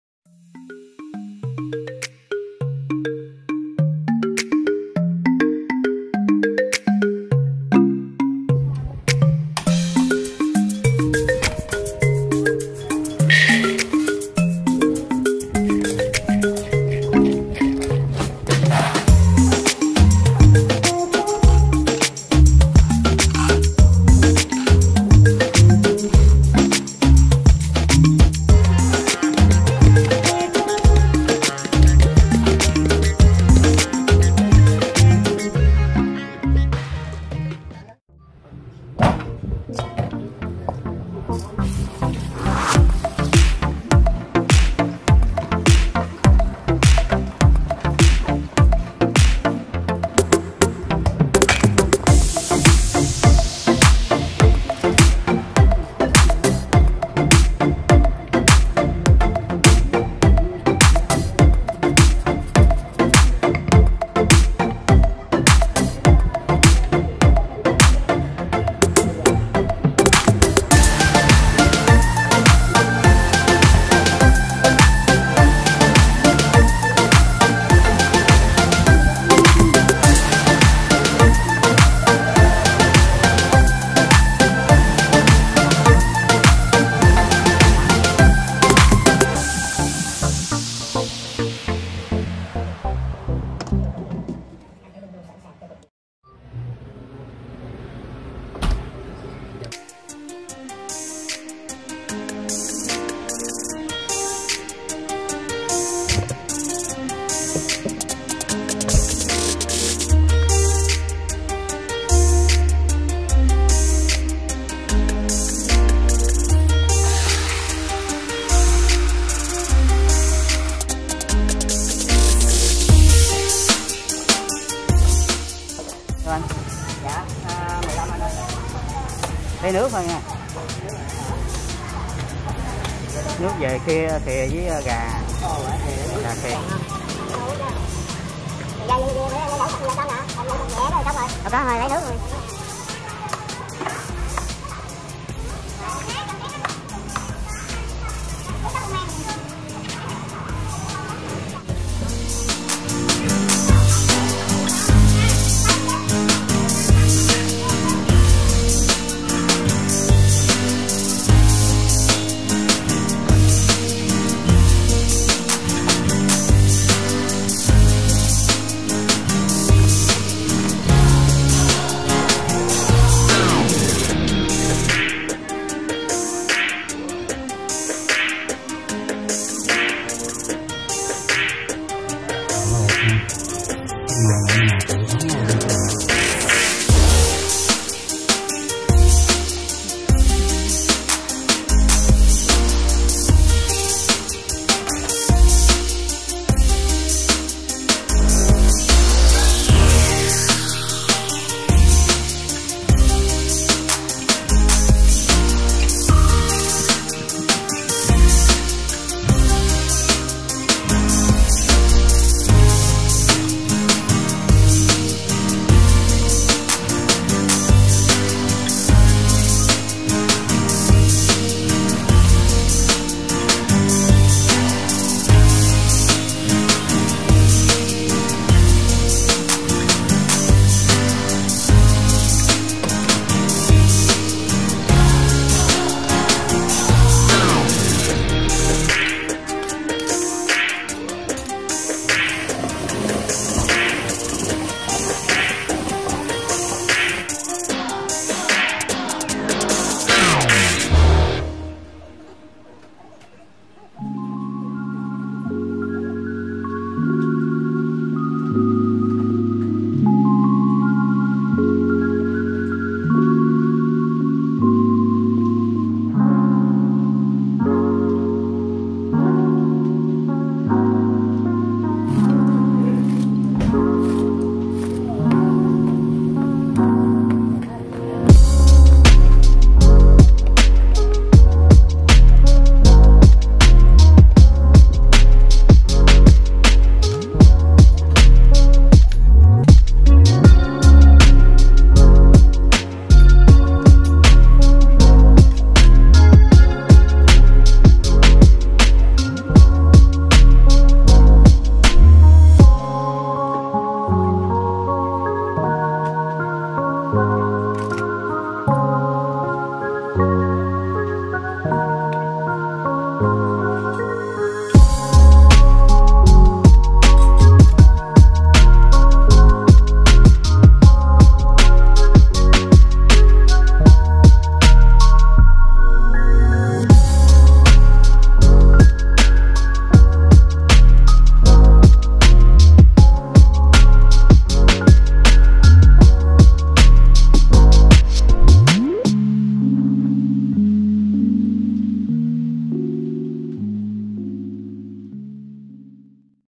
Xôi Gà Ta |Kênh A Ni #12
Xinh chào các bạn hôm nay mình chia sẽ món ăn Xôi Gà Ta một món ăn mà người dân miền tây rất thích.